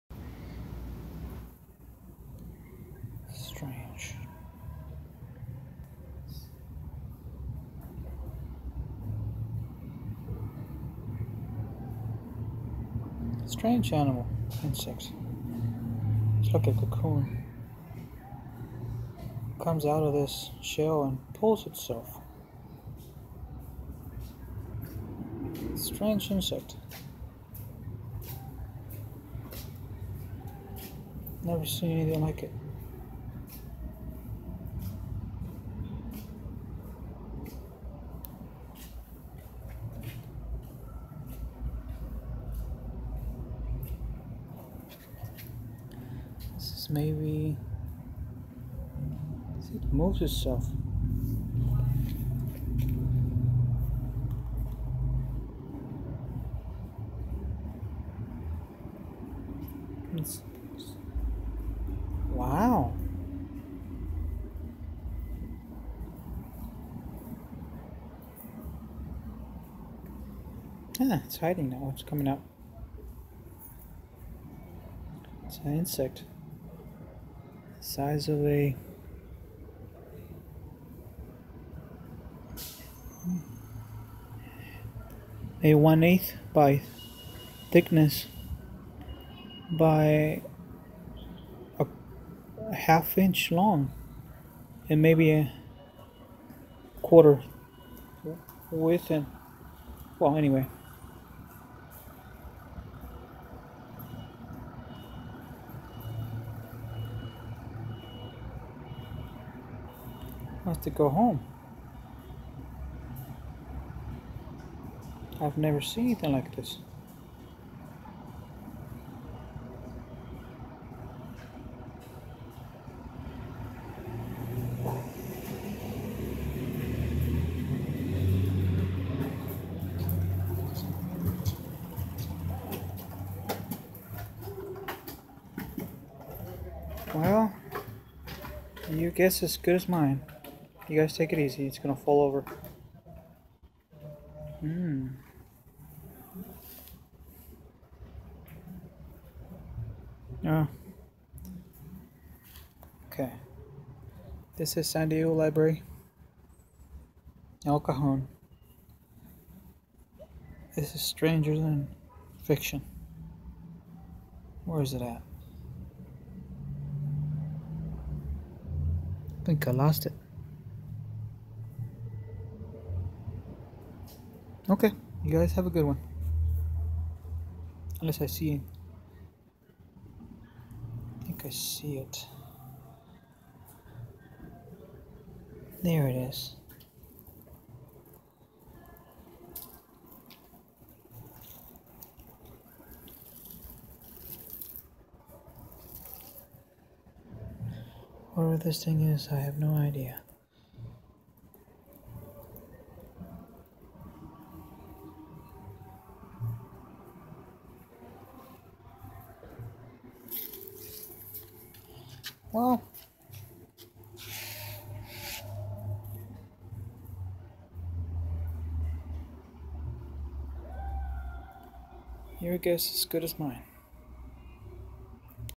SMART INSECT
Insect still resides in El Cajon Library , was put back safely back into its territory.